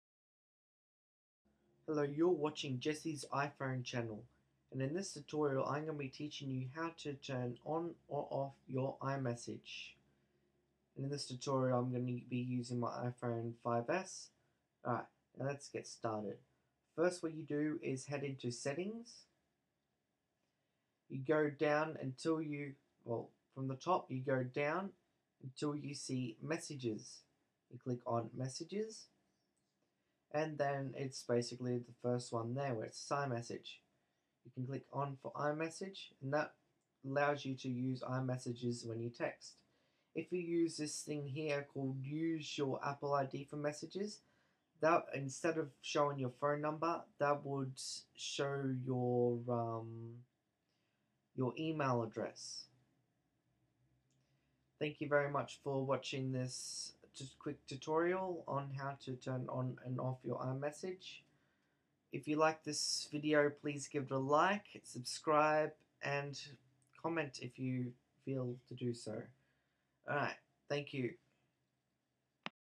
How to turn iMessage on/off
Have you wanted to turn on iMessage on your iPhone but didn't know how? Well in this tutorial, I will tell you exactly how to turn on or off your iMessage on your iPhone.

its basically quite simple to do, just follow these stapes also shown in the video
1) Open the settings settings app
2) Scroll down until you see messages, tap on that
3) It is the first one you should see displaying "iMessage", next to it should be a switch which you should be able to tap/slide to trigger it to turn on or off.

Incase you didn't know what the difference is between assign if you are sending a normal message or an iMessage, a normal message when sent by you appears green, and an iMessage when sent by you appears Blue, when someone has text you, both messaging services appear the same colour (grey), but it usually tells you above the first text of the conversation in small writing which messaging service you are using.

iMessage is a messaging service which can be used on most of apples products, these devices include the iPhone, iPad, iPod and even the MacBook Pro. the ID of the message is either the number of the sim card in the phone, or an email address that is associated with the users iTunes account.

if you want to change the ID showed when using the iMessage service, it is below where you turn the iMessage on/off, it is called Send & Receive, you click on that and you can choose either your email account or your phone number.

iMessage is a free texting service designed to allow you to send a free text from your apple device to another, it does however use data usage for every text sent, so make sure that you have enough data allowance for iMessage when you turn iMessage on. If you are on a plan where you have unlimited texts, it may be better to keep iMessage turned off to save extra data usage.

usually depending on your data connection speed, it is most likely that it would be faster to send a normal message then iMessage, especially if you are using the data on the phone with your carrier, although it should be ok if you are in a good area with high signal connection, or you are using wifi with a decent connection iMessage should be ok.

Another benefit with using iMessage is that when you send a text to whomever receives it, it shows the status of if that message has been delivered to their phone, if the message is read or if the receiver is sending you a reply back, just like how Facebook messaging works.

A disadvantage to iMessage is that you only can send messages using the iMessage service to other apple products (such as iPhones, iPads, iPods, and even MacBooks.) you cannot send an iMessage to a device that is not produced by Apple.

If this tutorial helped you, please subscribe and like this video. also comment in the moment section if you cannot how to figure out how to do something in your iPhone settings, and I will do my best to help you out!